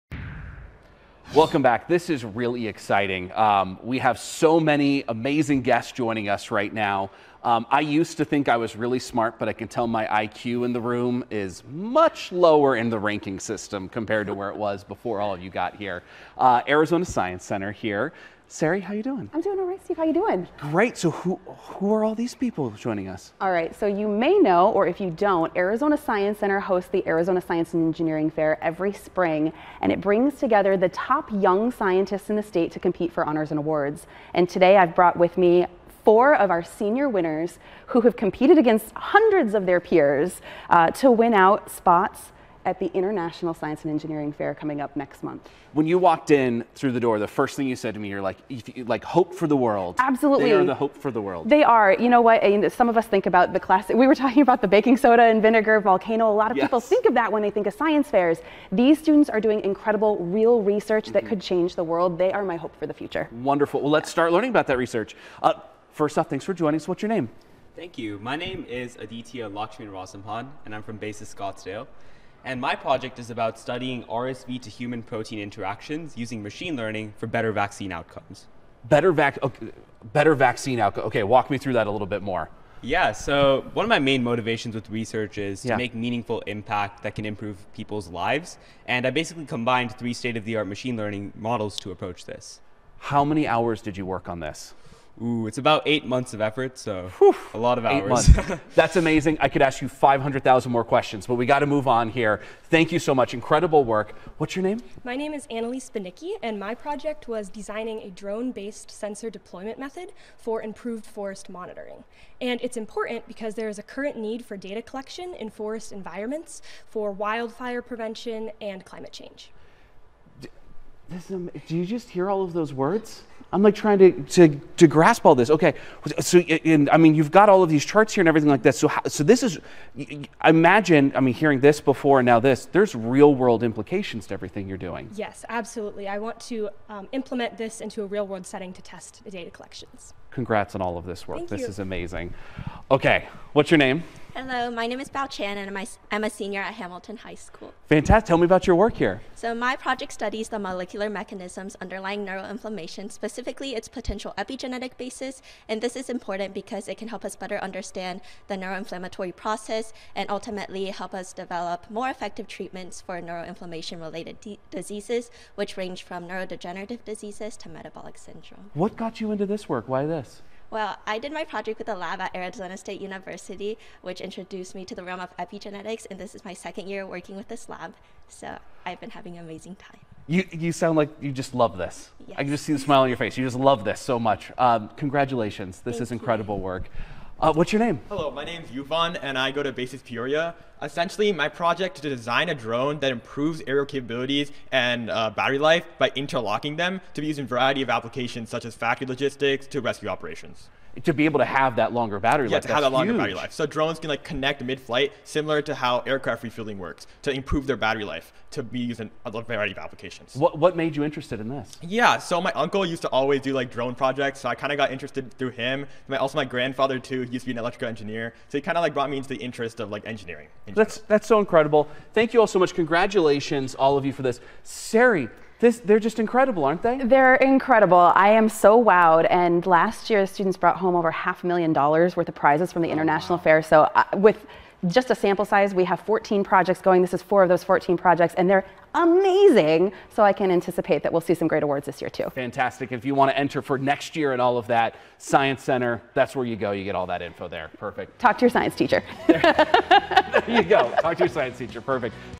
2024 Arizona Science and Engineering Fair (AzSEF) Grand Prize Winners
Celebrating the Future of STEM with 2024 AzSEF Grand Prize Winners.

Hear from FOUR of the Grand prize winners heading off to the Regeneron International Science and Engineering Fair (RISEF) in Los Angeles. Regeneron ISEF finalists compete for nearly $8 million in awards, prizes, and scholarships.

Baochan Fan
Hamilton High School
First Place: Senior - Cellular & Molecular
A novel identification of the epigenetic enzyme JMJD1a in neuroinflammation
Project Overview: Studying the molecular mechanisms underlying neuroinflammation and its importance because uncovering the potential epigenetic basis of neuroinflammation can help us understand the development of neuroinflammation-related diseases and lead us to new treatment paths.

Annalise Baniecki
ASU SCENE
First Place: Senior - Robotics & Intelligent Machines
Designing a Drone-Based Sensor Deployment for Improved Forest Monitoring
Project Overview: Lightweight yet durable launcher that attaches to a drone. The launcher shoots a projectile with a sensor that attaches to trees to monitor temperature and moisture, aiding in wildfire management and forest health.

Yuvan Senthil
BASIS Peoria
First Place: Senior - Engineering Technology: Statistics & Dynamics
I.D.A.S - Interlocking Drone Aerial Swarm: Enhancing Thrust and Battery Configuration Flexibility
Project Overview: Connecting in midflight to improve their thrust and battery life to be used in various applications, ranging from factory logistics to rescue operations.

Aditya Lakshminarasimhan
BASIS Scottsdale
First Place: Senior - Computational Biology & Bioinforatics
Elucidating RSV F-Fusion Protein-Host Interactions: Graph Attention Networks (GAN) and Structural Visualization
Project Overview: Visualizing the RSV-Human protein interaction using Machine Learning for better Vaccine Outcomes.